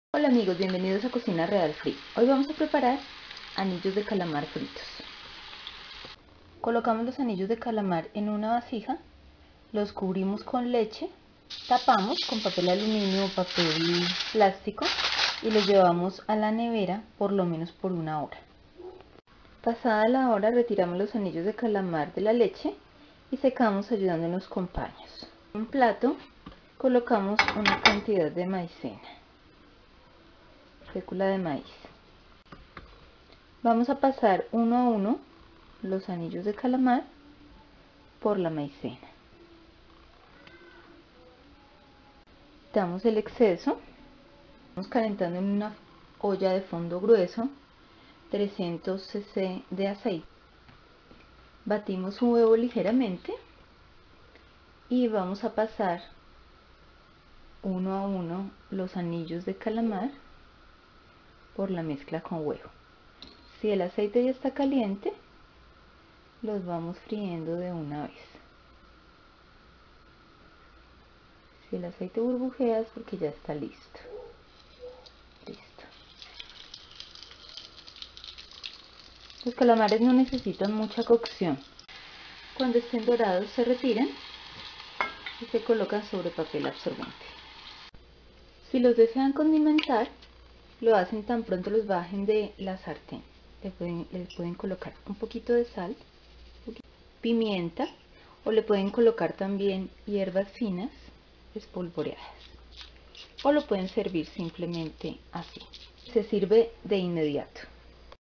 Anillos de Calamar Fritos
Los anillos de calamar son económicos y pueden prepararse de diversas formas, fritos son una excelente y rica entrada. Si los colocamos a remojar en leche una hora antes de freírlos, nos quedarán blandos y jugosos por dentro y crocantes y dorados por fuera. También los pueden servir con pasta  o ensalada. Los invito a prepararlos.